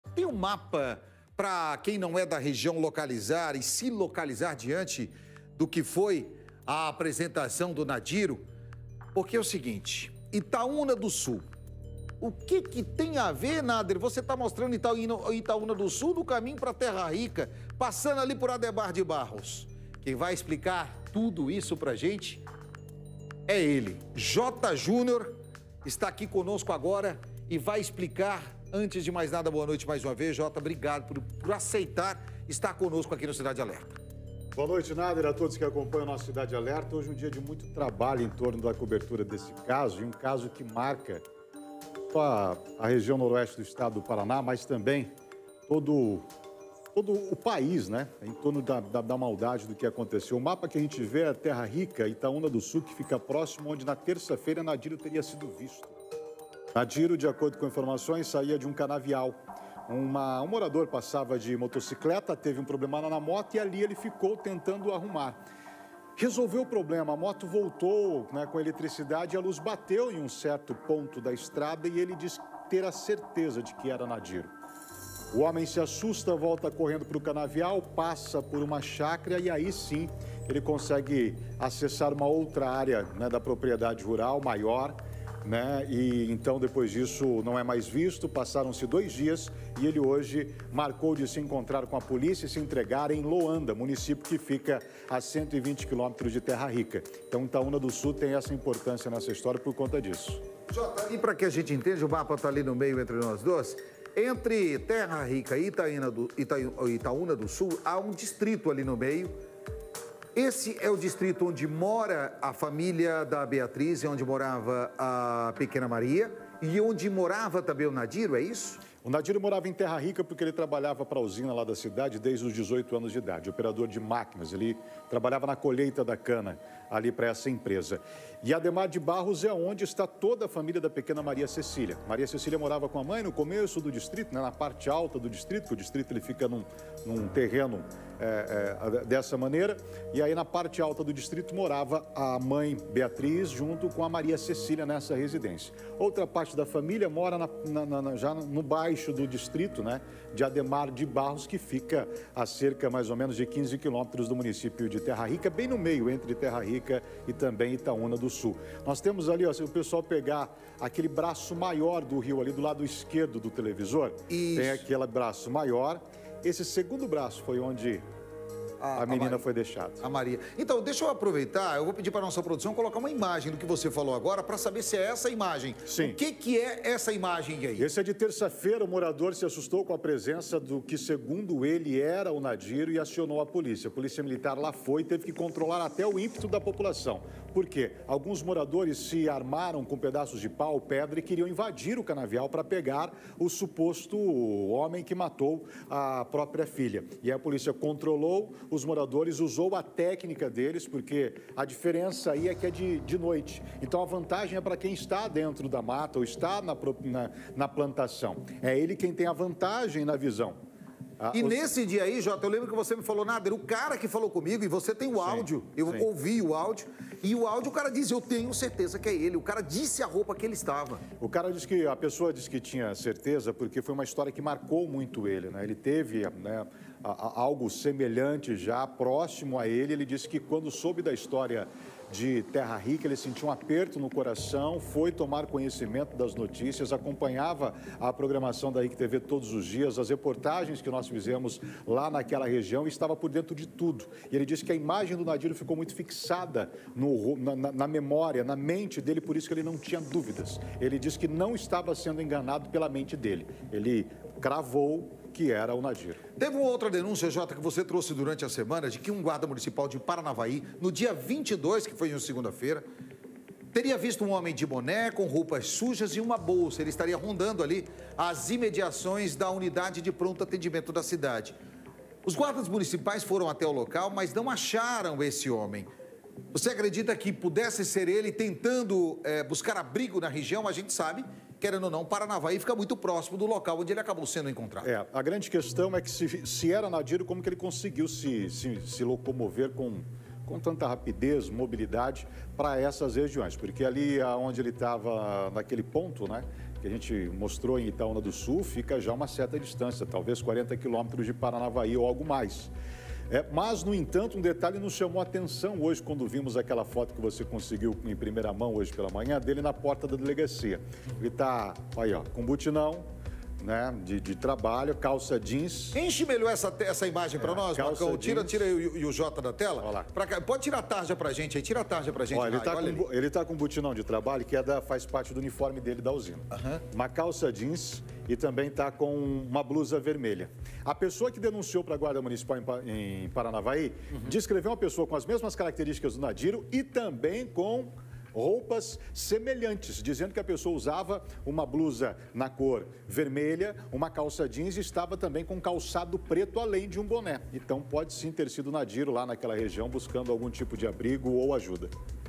Moradores da região de Itaúna já tinham avisado a polícia que o suspeito estava escondido em canavia
Moradores da região de Itaúna já tinham avisado a polícia que o suspeito estava escondido em canavial.